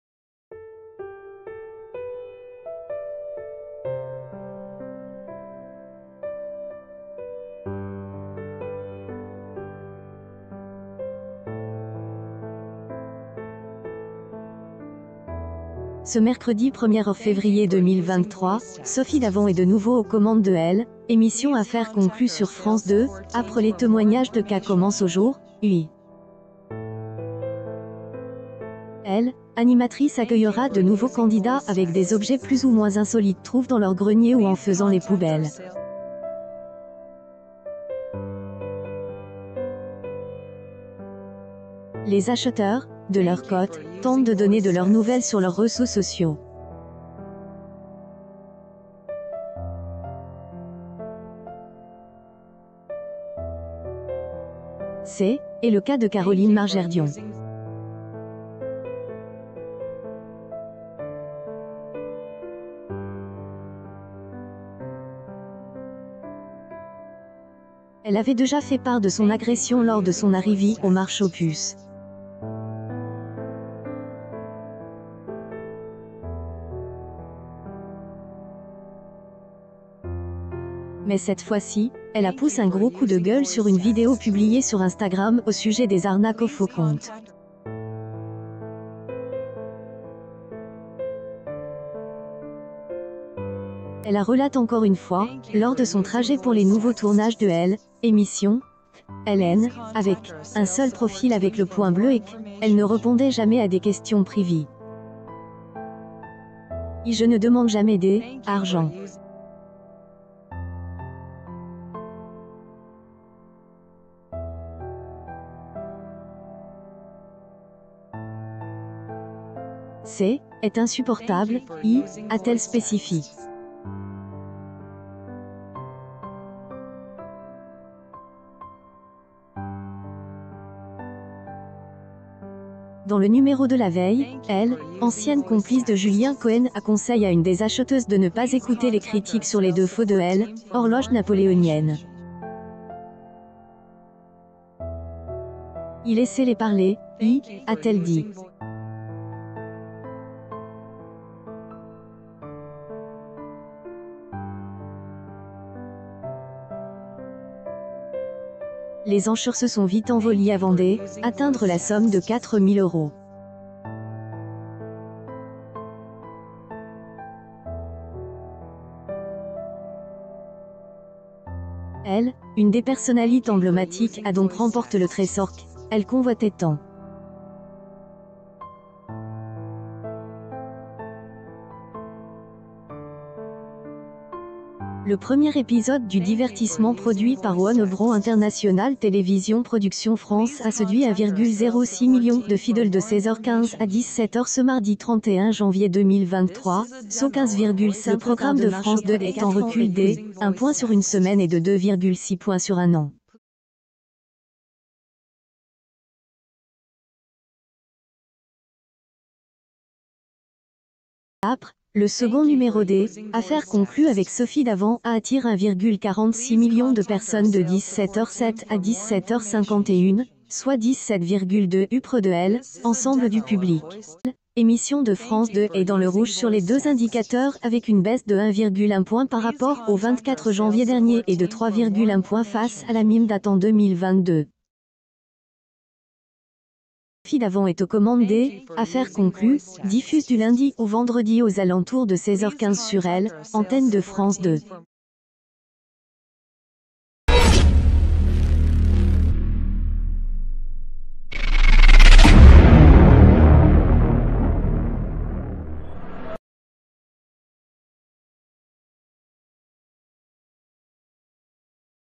Affaire conclue : “C’est insupportable !” Caroline Margeridon s’énerve après une arnaque sur Franc
Affaire conclue : “C’est insupportable !” Caroline Margeridon s’énerve après une arnaque sur Franc |Invicta Men's Pro Diver Stainless Steel Quartz Watch . Please help me with an order to maintain the system, thanks a lot.| Ce mercredi 1er février 2023, Sophie Davant est de nouveau aux commandes de l’émission Affaire conclue sur France 2, après les témoignages de Ça commence aujourd’hui . L’animatrice accueillera de nouveaux candidats avec des objets plus ou moins insolites trouvés dans leur grenier ou en faisant les poubelles. C’est le cas de Caroline Margerdion. Elle avait déjà fait part de son agression lors de son arrivée au marché aux puces. Mais cette fois-ci, elle a poussé un gros coup de gueule sur une vidéo publiée sur Instagram au sujet des arnaques aux faux comptes. Elle a relaté encore une fois, lors de son trajet pour les nouveaux tournages de l’émission, qu’elle n’avait qu’un seul profil avec le point bleu et qu’elle ne répondait jamais à des questions privées. « Je ne demande jamais d’argent. C’est insupportable », a-t-elle spécifié. Dans le numéro de la veille, l’ancienne complice de Julien Cohen a conseillé à une des acheteuses de ne pas écouter les critiques sur les défauts de l’horloge napoléonienne. « Laissez-les parler », a-t-elle dit. Les enchères se sont vite envolées avant d’atteindre la somme de 4000 euros. L’une des personnalités emblématiques a donc remporté le trésor qu’elle convoitait tant. Le premier épisode du divertissement produit par Warner Bros International Télévision Production France a séduit 1,06 million de fidèles de 16h15 à 17h ce mardi 31 janvier 2023, sot 15,5% de part de marché auprès des quatre ans et plus. Le programme de France 2 est en recul d’un point sur une semaine et de 2,6 points sur un an. Juste après, le second numéro d’ Affaire conclue avec Sophie Davant a attiré 1,46 million de personnes de 17h07 à 17h51, soit 17,2% auprès de l’ensemble du public. L’émission de France 2 est dans le rouge sur les deux indicateurs avec une baisse de 1,1 point par rapport au 24 janvier dernier et de 3,1 points face à la même date en 2022. Sophie Davant est aux commandes d’ Affaire conclue , diffusée du lundi au vendredi aux alentours de 16h15 sur l’antenne de France 2.